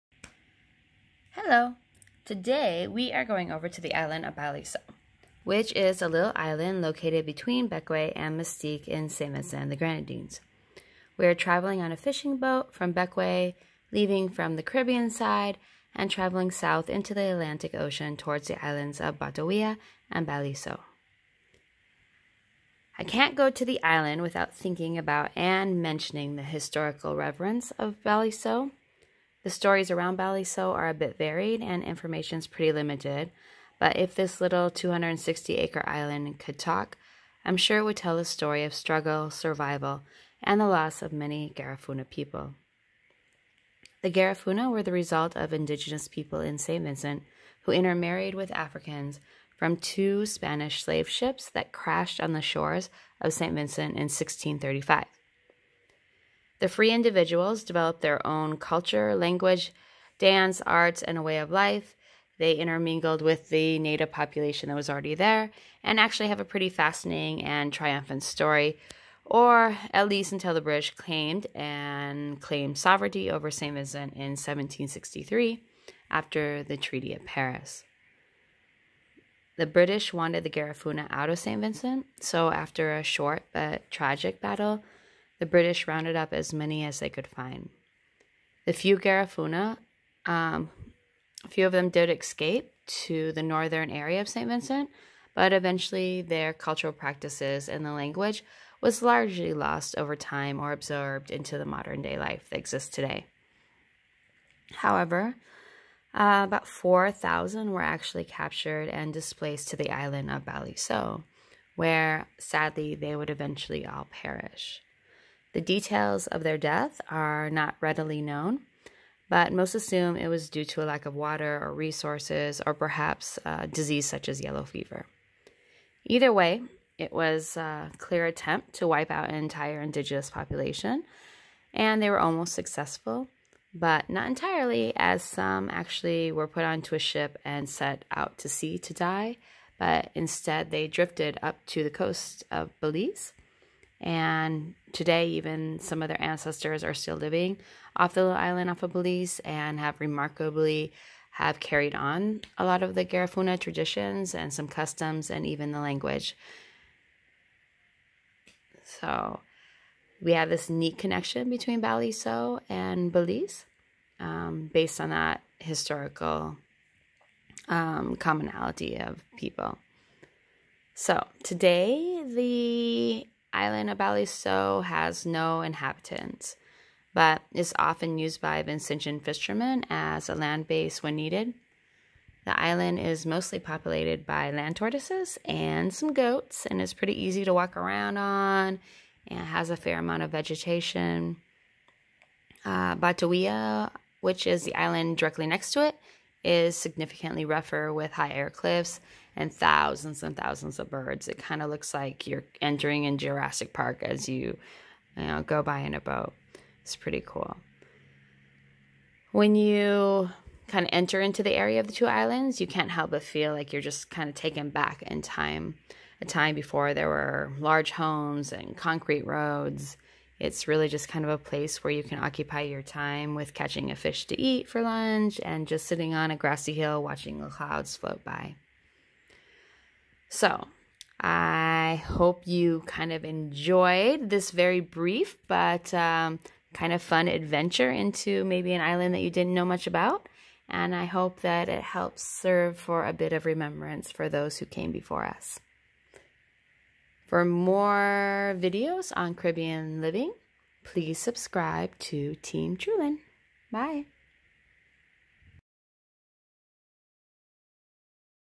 Balliceaux
A brief story of the Garifuna and the island of Balliceaux in St. Vincent and the Grenadines.